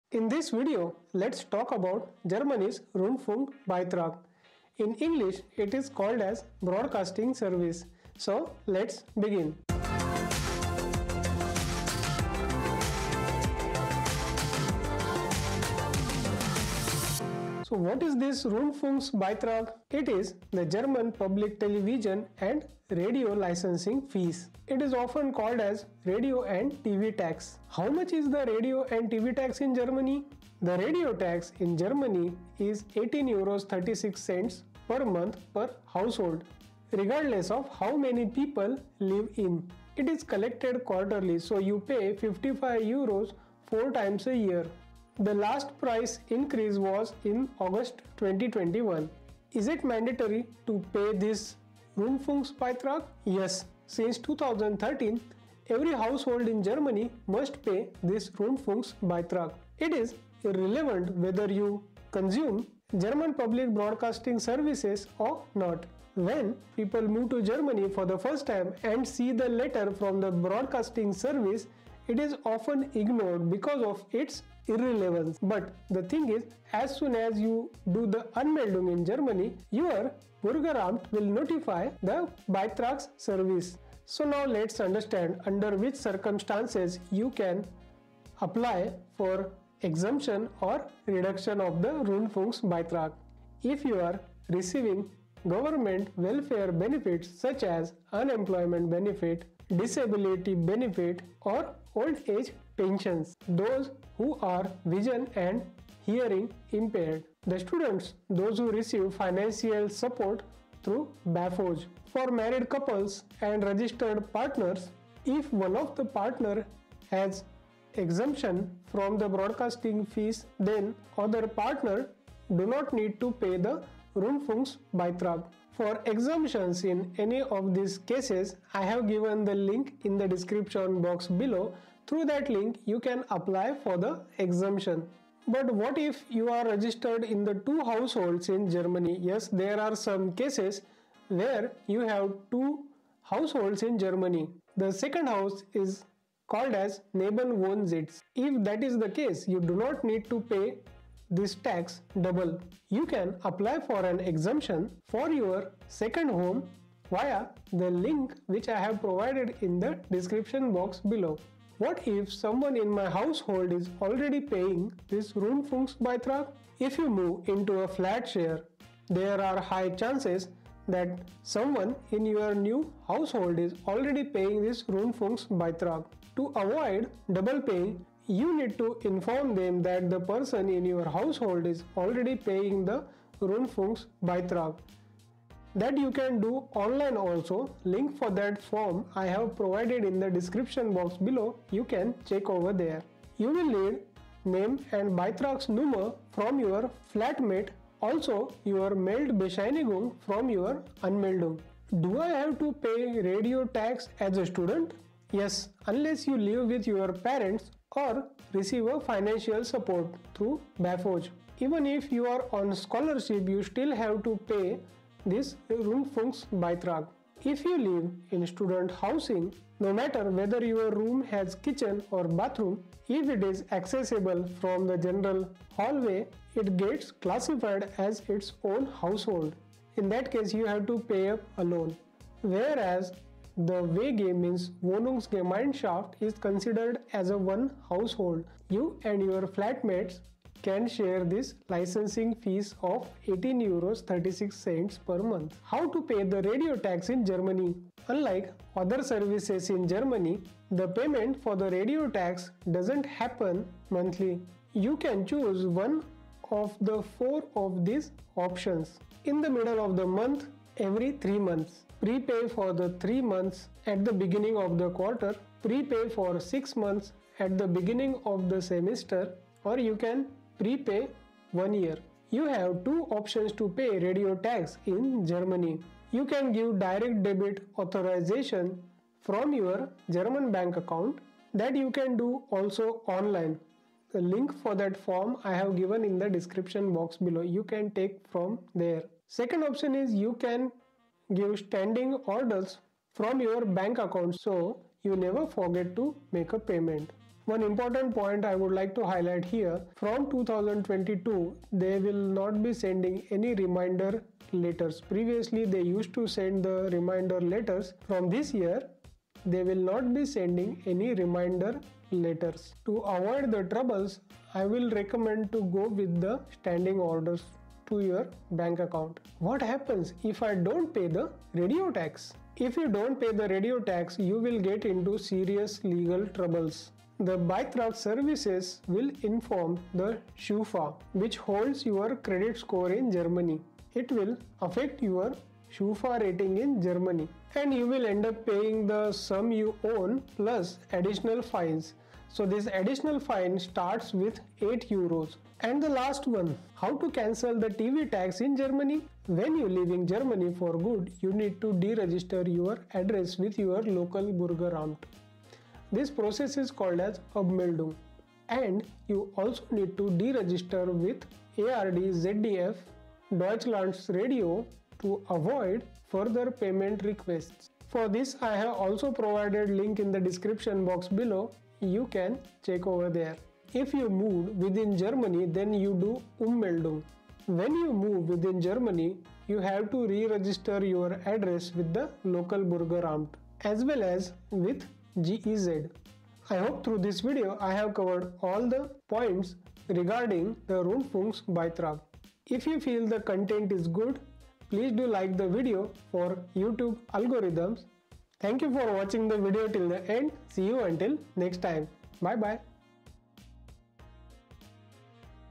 Do I Have to pay Rundfunkbeitrag? | Broadcasting service of Germany [ENGLISH]
In this video, let’s talk about Germany’s Rundfunkbeitrag. In English, It’s called Broadcasting service. Due to inflation and the current economic situation, Germany's monthly TV license fee of €18.36 / month can be a burdensome expense. But, there are some circumstances where you don’t have to pay.

Key Takeaways
- What is Rundfunkbeitrag
- How much does it cost?
- Is it mandatory to pay the Rundfunkbeitrag
- Under which circumstances can you apply for an exemption
-  Registered in two households in Germany
- What if someone is paying in my household (Flat share)
- Do I have to pay as a Student?
- How to pay for Radiotax in Germany?
- What happens if you don't pay Radio tax?
- How to cancel Radiotax in Germany?